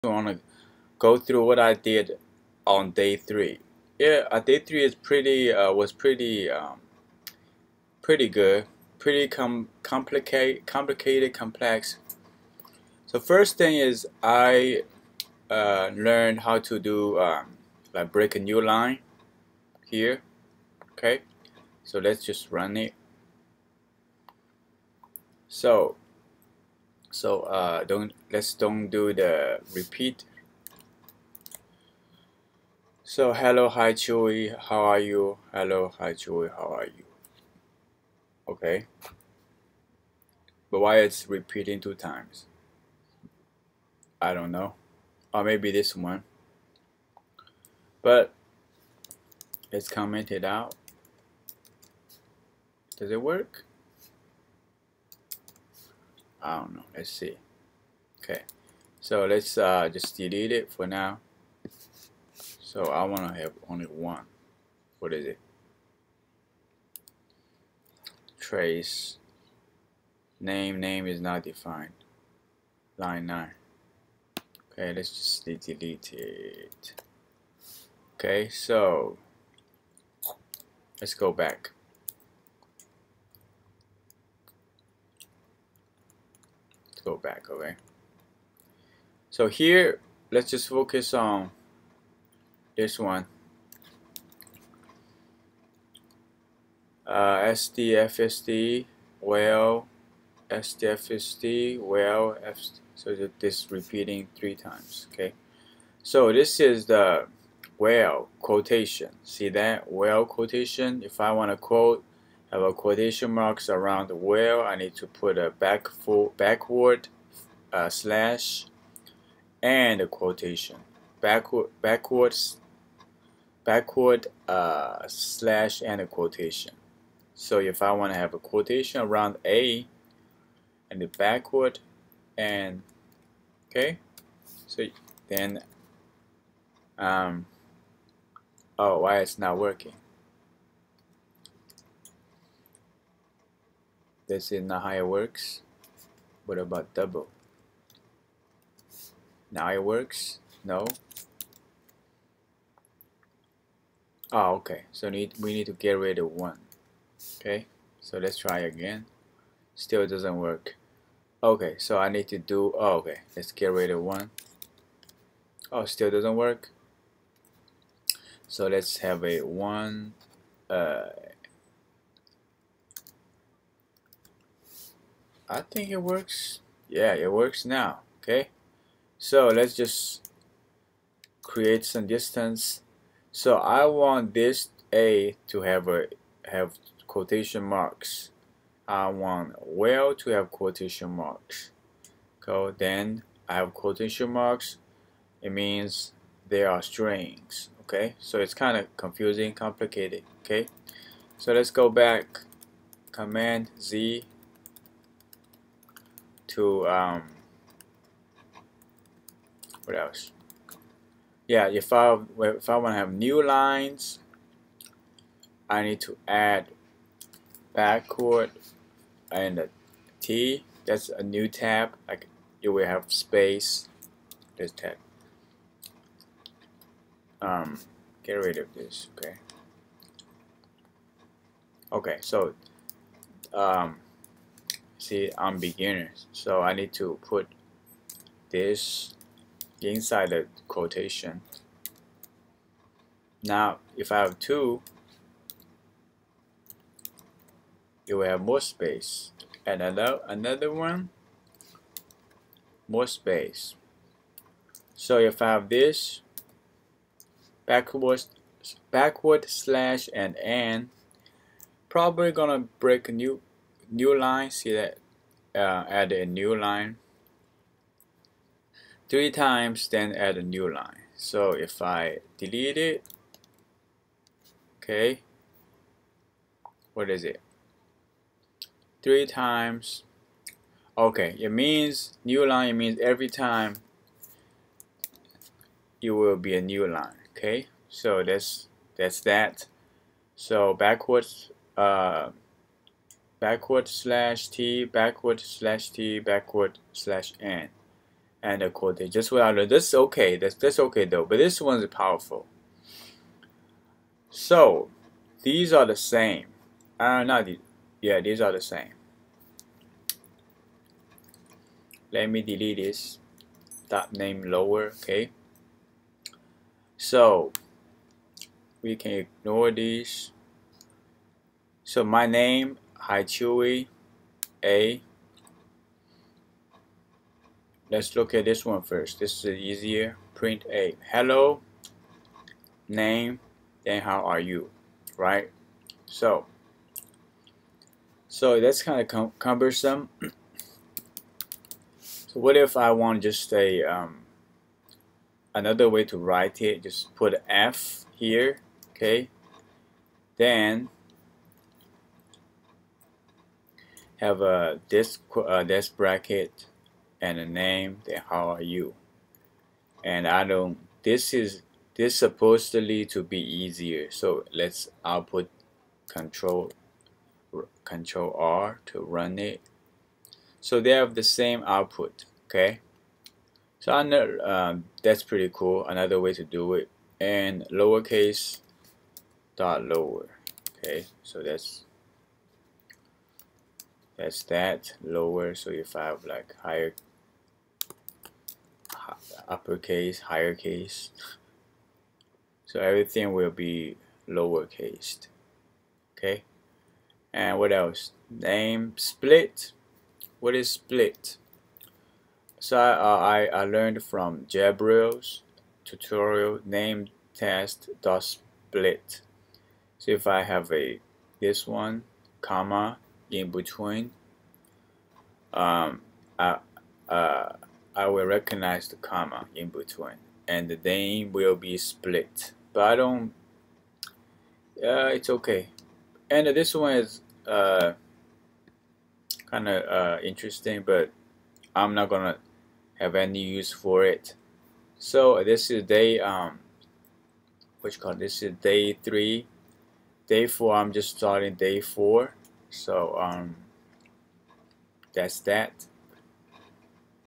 Day 4, recap of day 3 Coding in Python. \n \"
I will spend 5 mins or even just one min to learn programming for the next 100 days. I'm a complete beginner.